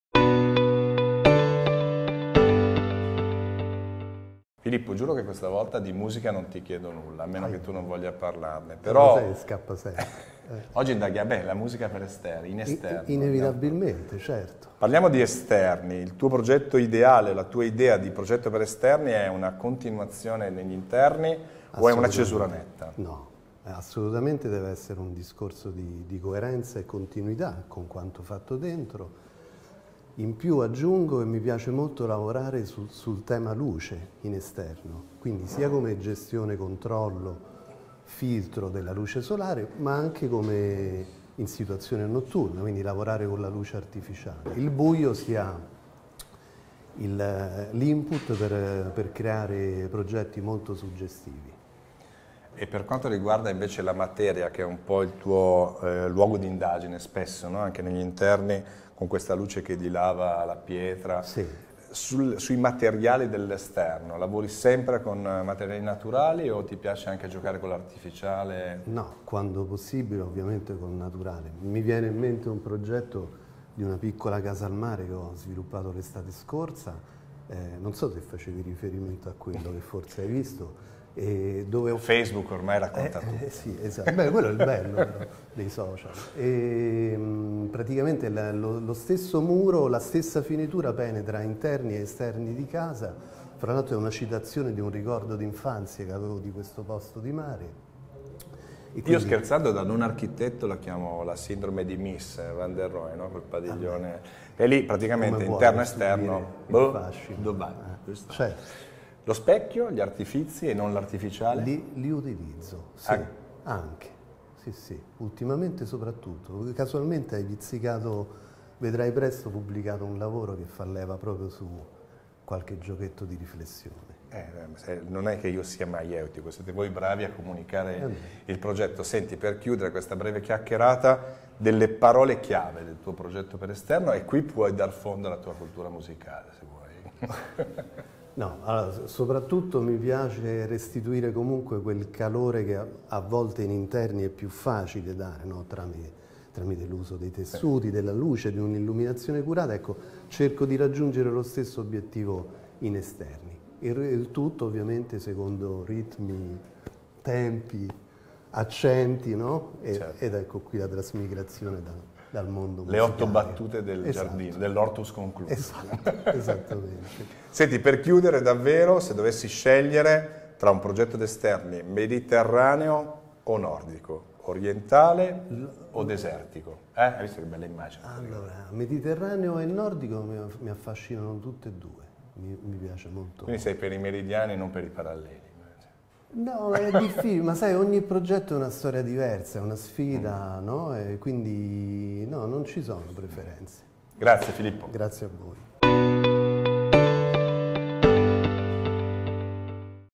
Corradi Outdoor Attitude Bologna - Giorgio Tartaro intervista l'architetto Filippo Bombace
L'architetto Filippo Bombace (filippobombace.com): luce, materiali e armonia tra interno ed esterno.